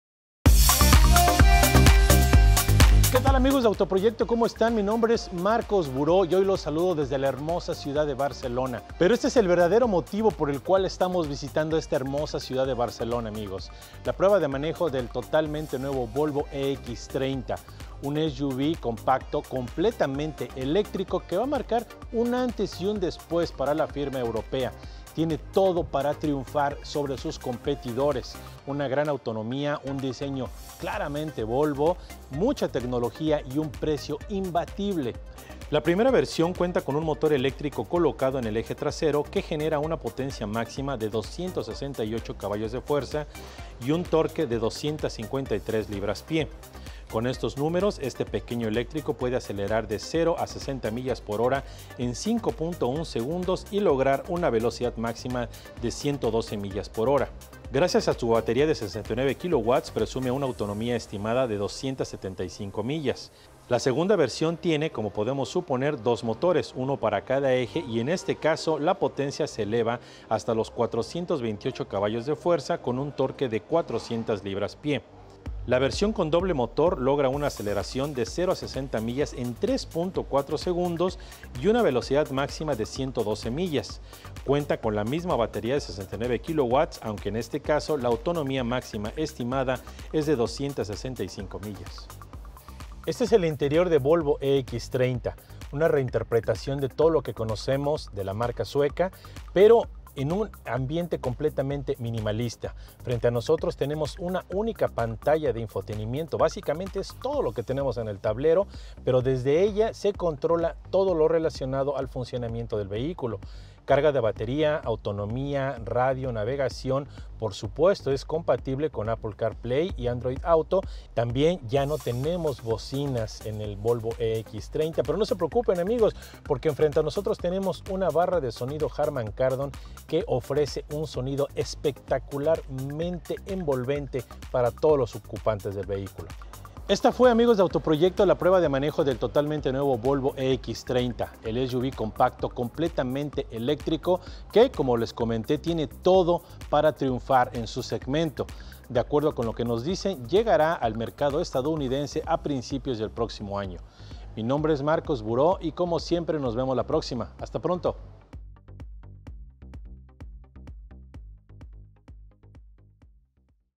Volvo EX30 2025, prueba de manejo en Barcelona
En esta ocasión, gracias a una invitación de la marca sueca, dimos el salto y atravesando el océano llegamos hasta Barcelona, para formar parte de un amplio grupo de periodistas provenientes de todas part5es del mundo. ¿El motivo? La prueba de manejo del totalmente nuevo Volvo EX30 2025. Un vehículo que está marcando un antes y un después en la trayectoria de la firma que ahora es propiedad de Grupo Geely.
Visita nuestro sitio web para estar al tanto de todo lo que acontece en el mundo automotriz